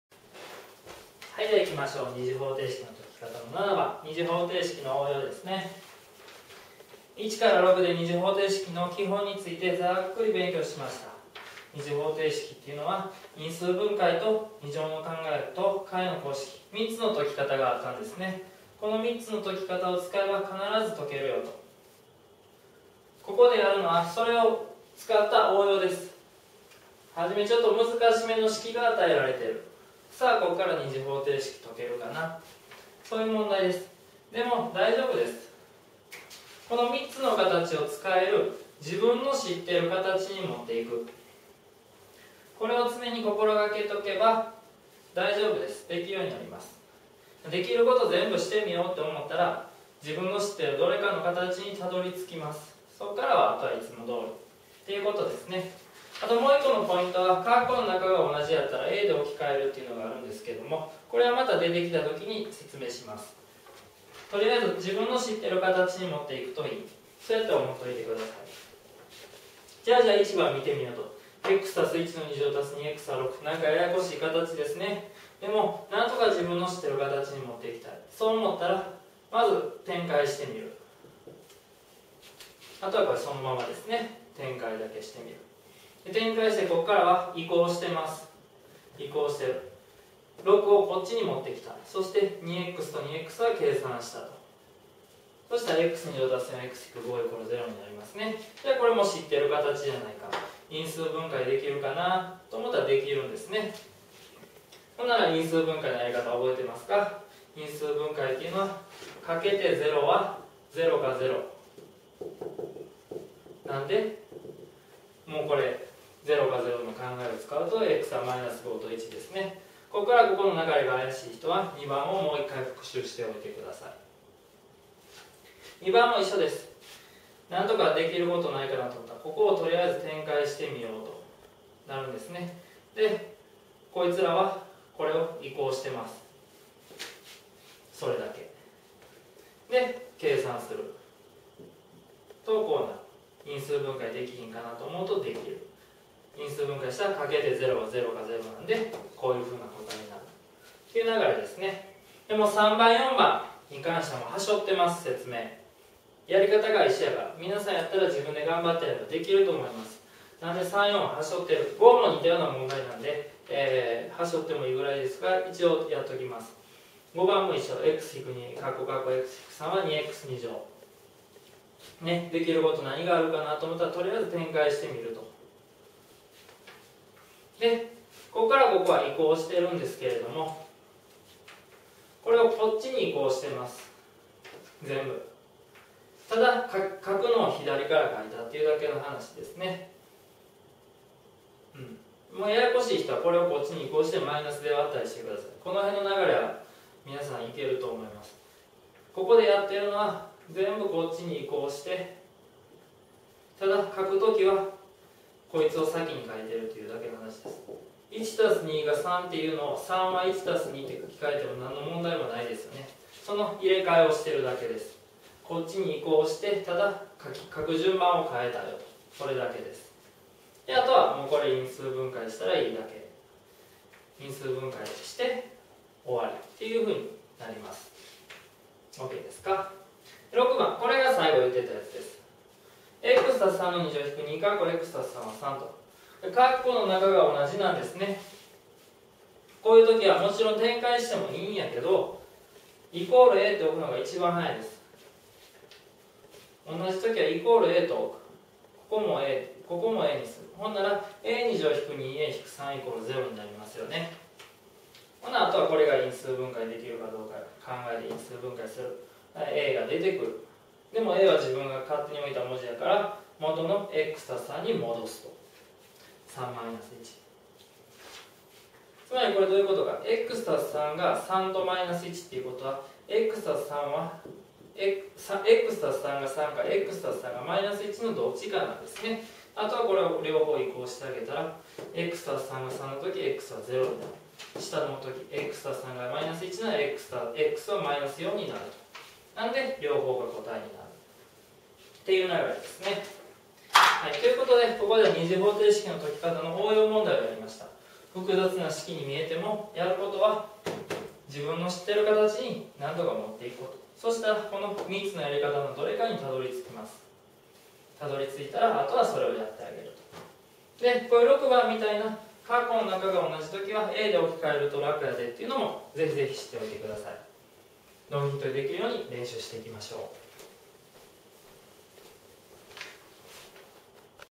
二次方程式の応用（中３数学：二次方程式）
入試や定期テストによく出る問題を紹介、解説しています。